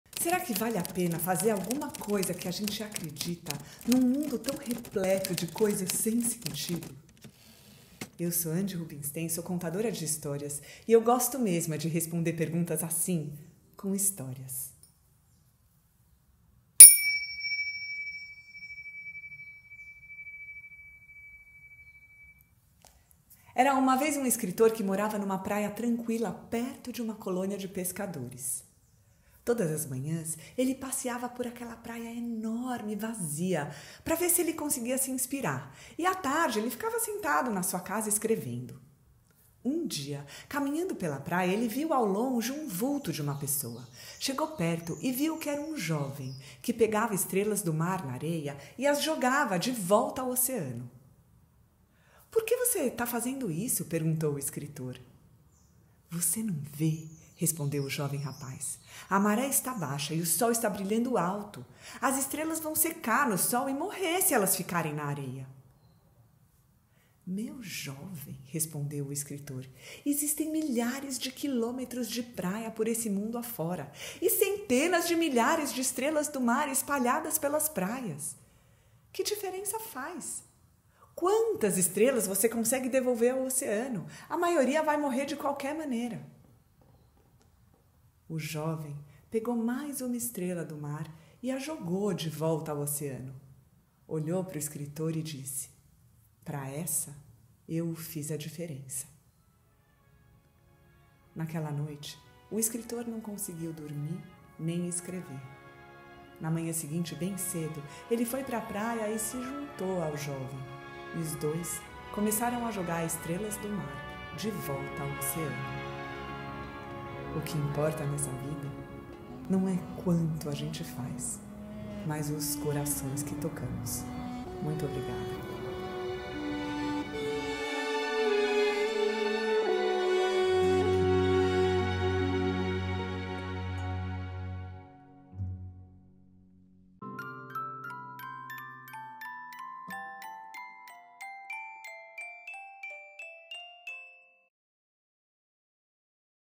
O Rapaz e a Estrela do Mar - Andi Rubinstein | Contação de Histórias para Adultos
Eu sempre digo:
— O que importa nesta vida, são os corações que tocamos.

O Rapaz e a Estrela do Mar

Conto do livro: Storytelling, contando histórias nas empresas
Autora: Gyslaine Avelar Matos
Narração: Andi Rubinstein
Edição: Juliano Garrett

Quer ouvir mais histórias?  Uma nova história toda terça feira às 7h15.